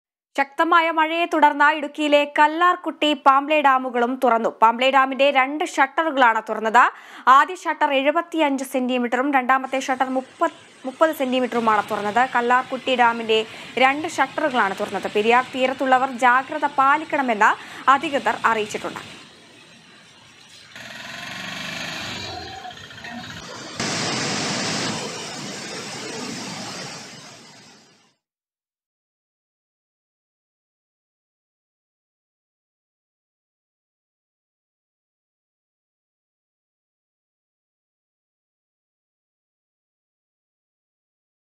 പെരിയാര്‍ തീരത്തുള്ളവര്‍ ജാഗ്രത പാലിക്കണം; ഇടുക്കിയിലെ കല്ലാര്‍കുട്ടി,പാംപ്ല ഡാമുകള്‍ തുറന്നു
ശക്തമായ മഴയെത്തുടര്‍ന്ന് ഇടുക്കിയിലെ കല്ലാര്‍കുട്ടി,പാംബ്ല ഡാമുകളും തുറന്നു. പാംബ്ല ഡാമിന്റെ രണ്ട് ഷട്ടറുകളാണ് തുറന്നത്. ആദ്യ ഷട്ടര്‍ 75 സെന്റീമീറ്ററും രണ്ടാമത്തെ ഷട്ടര്‍ 30 സെന്റീമീറ്ററുമാണ് തുറന്നത്. കല്ലാര്‍കുട്ടി ഡാമിന്റെ രണ്ട് ഷട്ടറുകളാണ് തുറന്നത്. പെരിയാര്‍ തീരത്തുള്ളവര്‍ ജാഗ്രത പാലിക്കണമെന്ന് അധികൃതര്‍ അറിയിച്ചു.

About COA